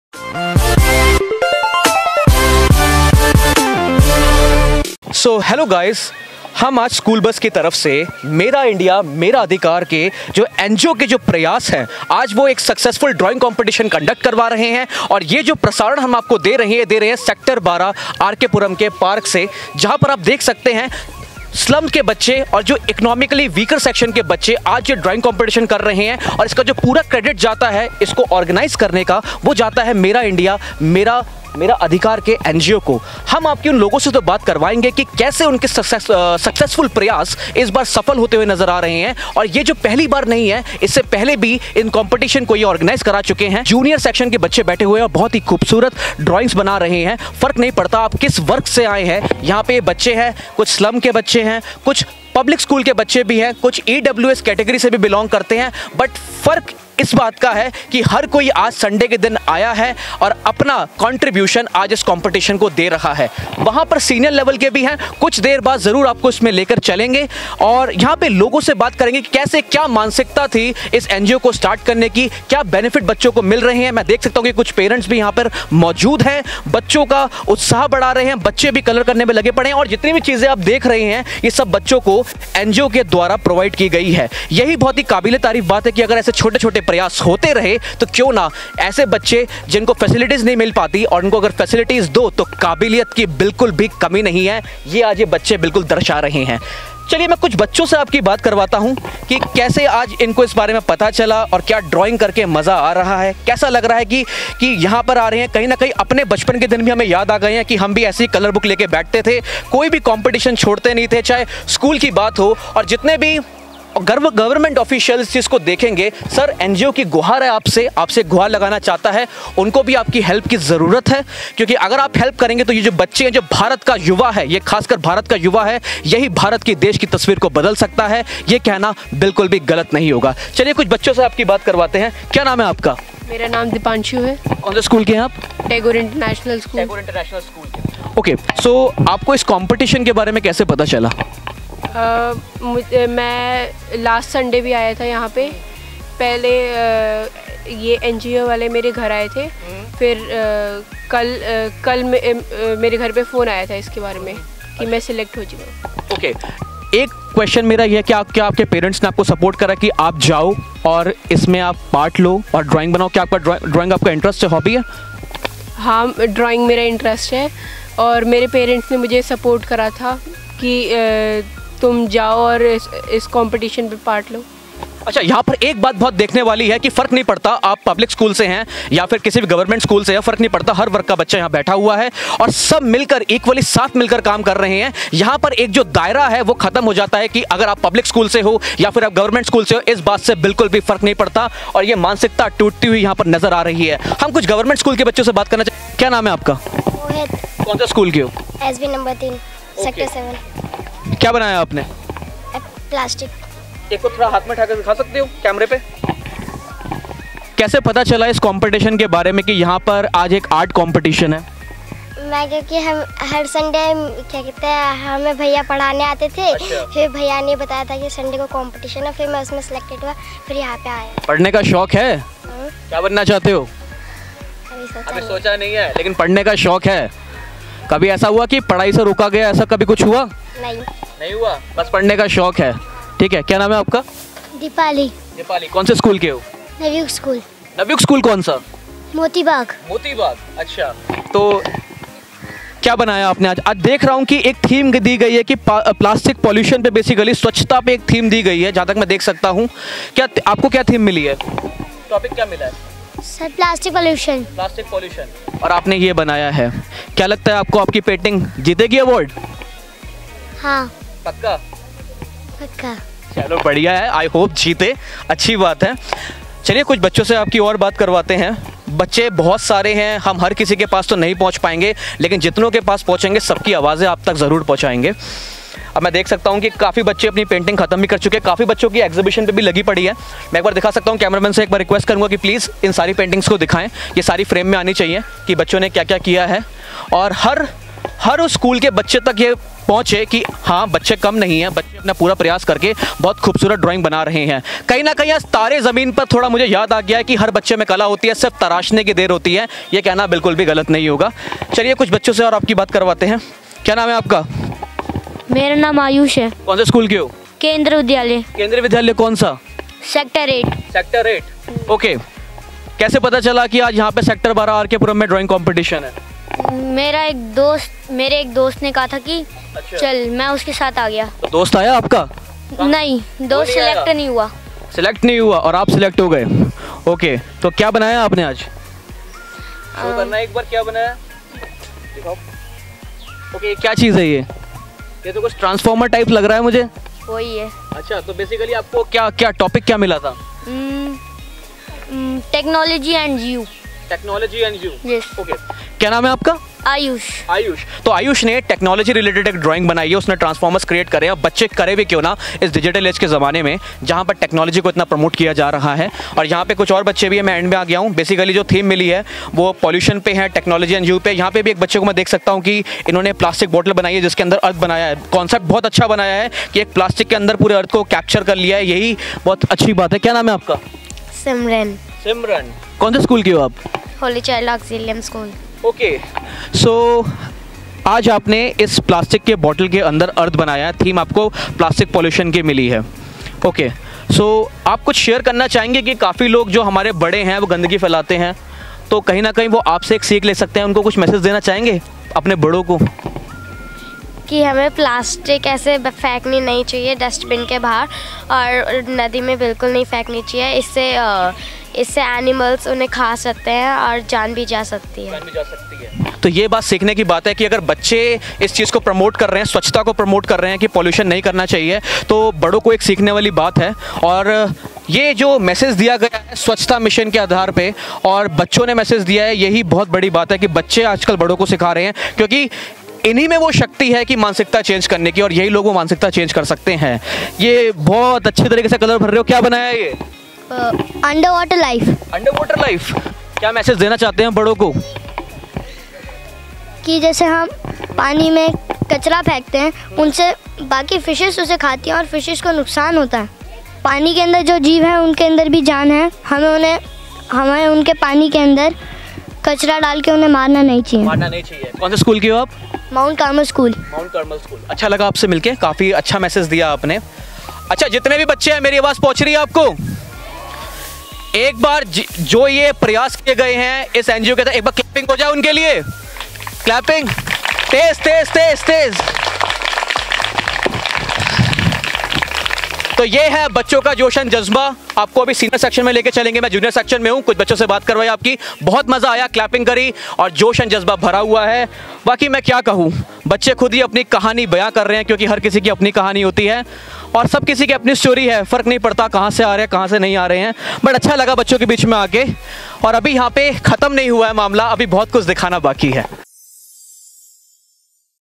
MIMA, an NGO is changing the lives of Slum children and the community for a better future
Mera India Mera Adhikar is a Registered Non- Government Organisation (Regd). NGO work is concentrated on 3 of the Fundamental Rights: Right to Education, Right to Equality and Right against exploitation, which we believe are the areas of greatest need and ensuring fulfillment of these rights is the focus of our organization.